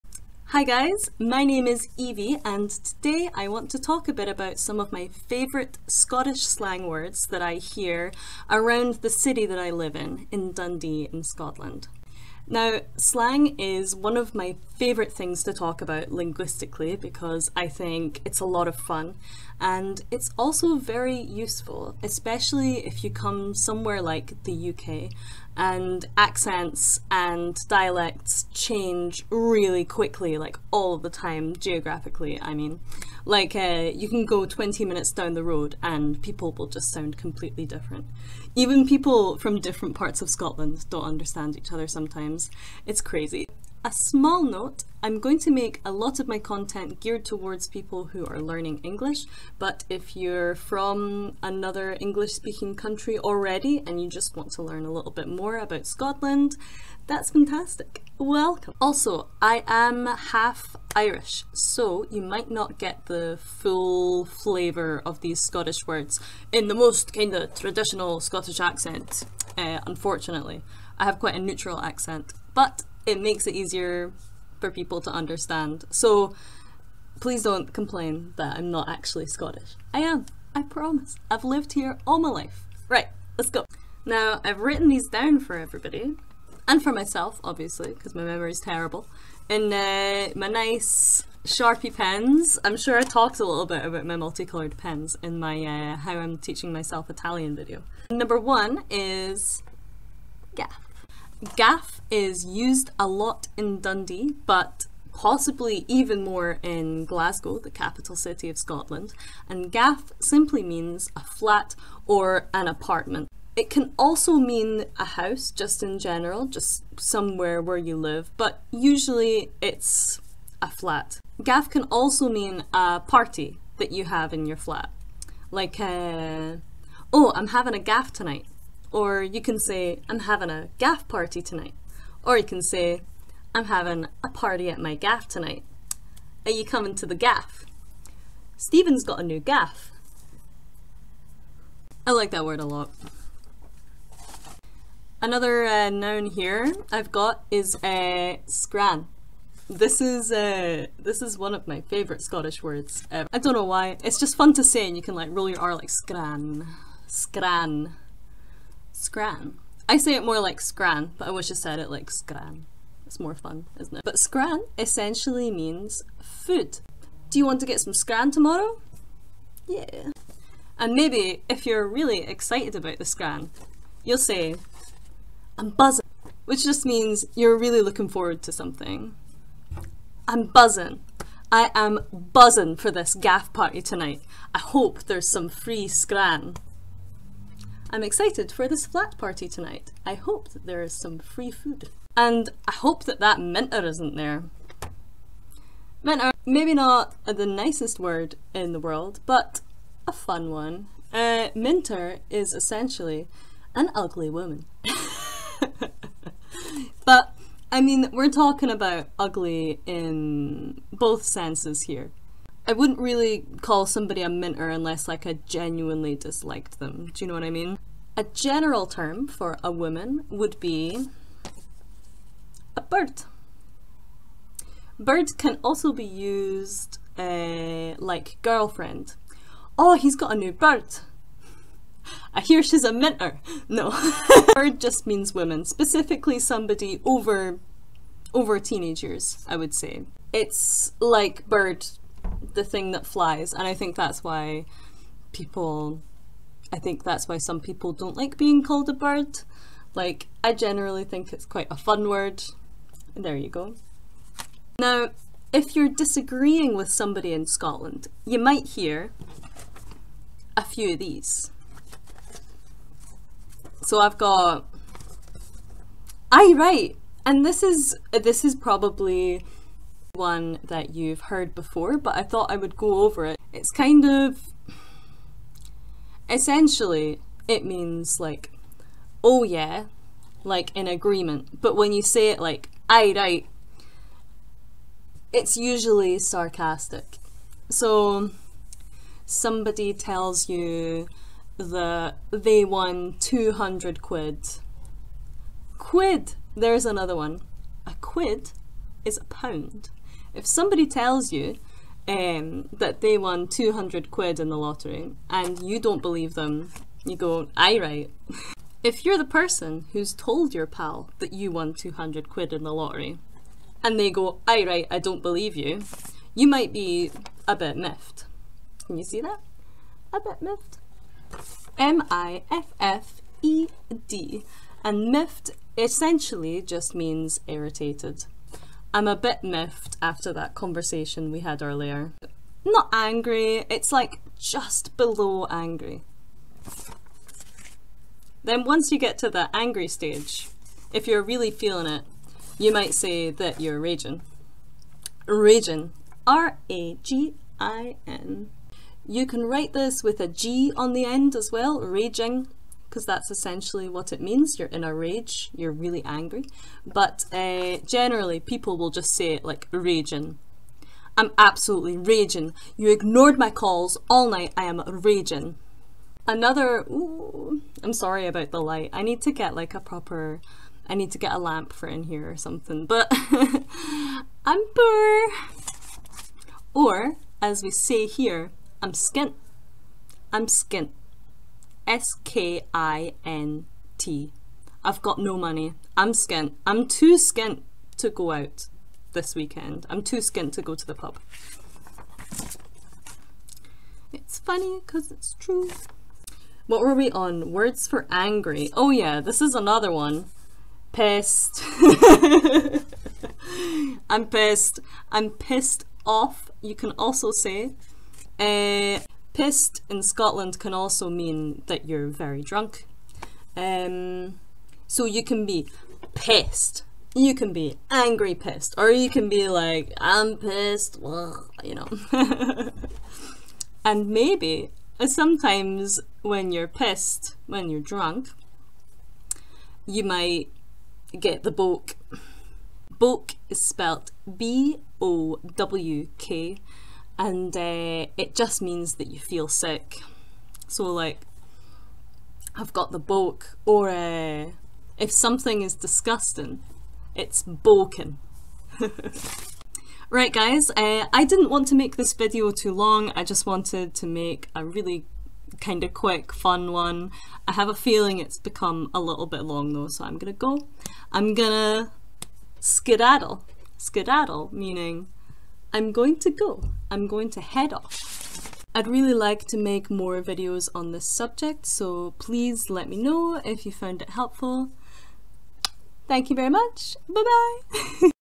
QUICK SCOTTISH SLANG
!! THE CAPITAL CITY IS EDINBURGH NOT GLASGOW SORRY!! sono stupida ogni tanto haha
Some useful and fun Scottish slang words! Fully subtitled in English